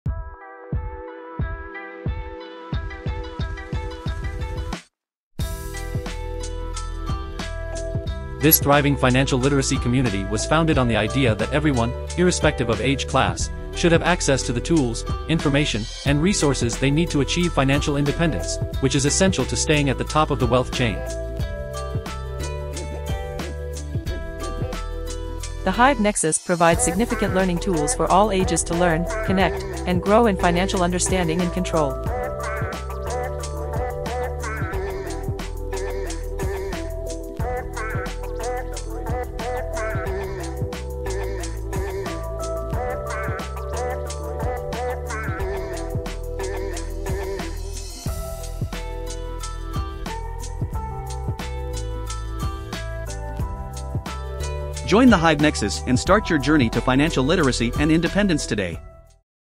THE HIVE NEXUS COMMUNITY TRAILER
A 1 minute video trailer on what the hive nexus community is all about, don't miss this